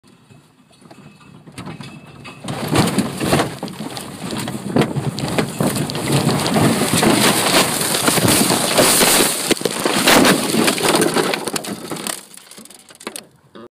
Peugeot 206 roll over in-car
Rolled 1 and a half times ended up on it's roof but my phone fell out as it was only jammed under the passenger head rest sorry it's a bit shit but I thort I mite as well up load it anyway